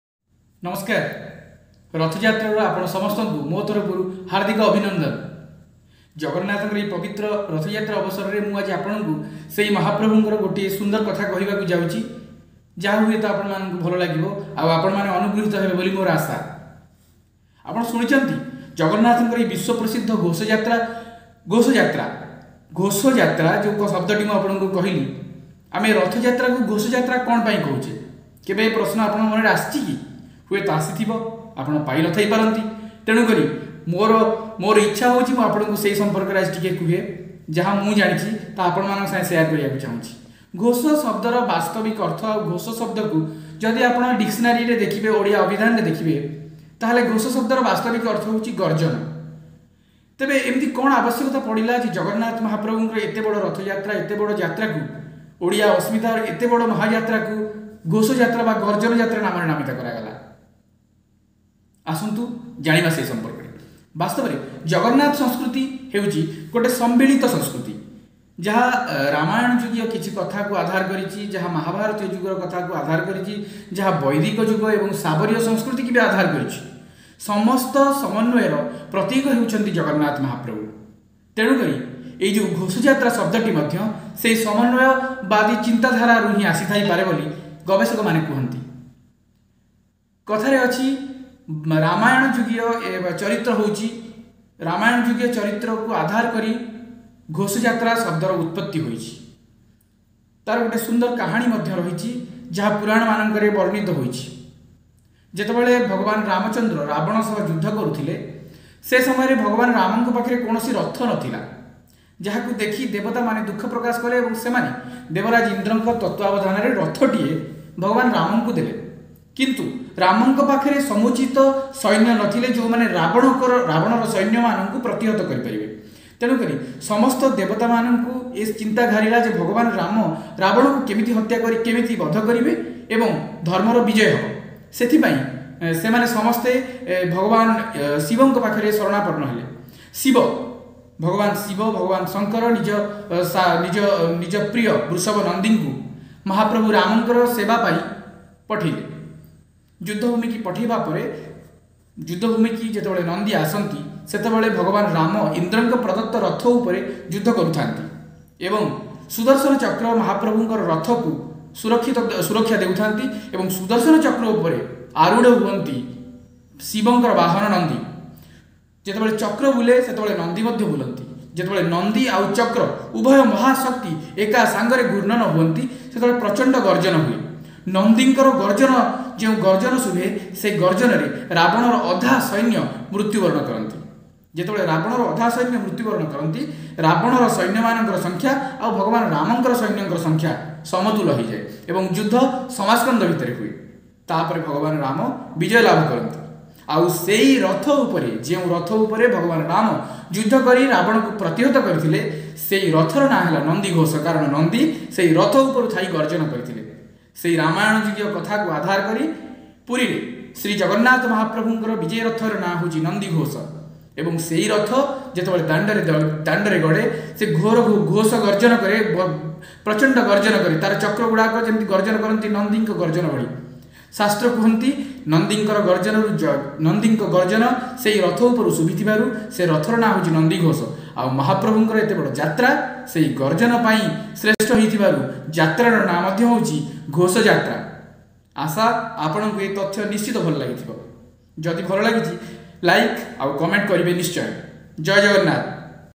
June 23, 2020
ଜଗନ୍ନାଥଙ୍କ ରଥଯାତ୍ରାକୁ କଣ ପାଇଁ ଘୋଷଯାତ୍ରା କୁହାଯାଏ ?